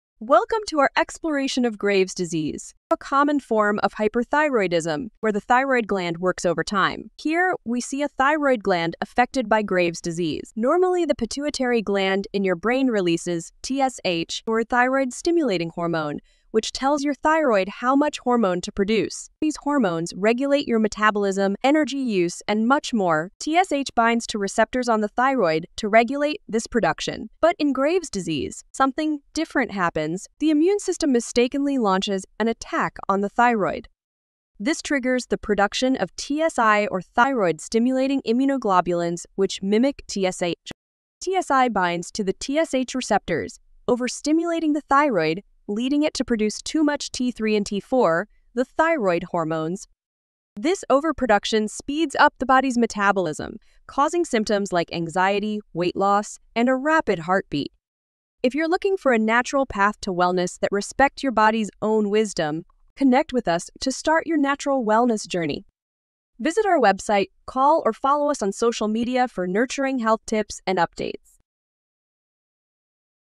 Understanding Graves' Disease
Graves disease is an autoimmune disorder that often causes hyperthyroidism, or an overactive thyroid. This condition leads to the excessive production of thyroid hormones, triggering symptoms such as rapid weight loss, heightened anxiety, and heart palpitations.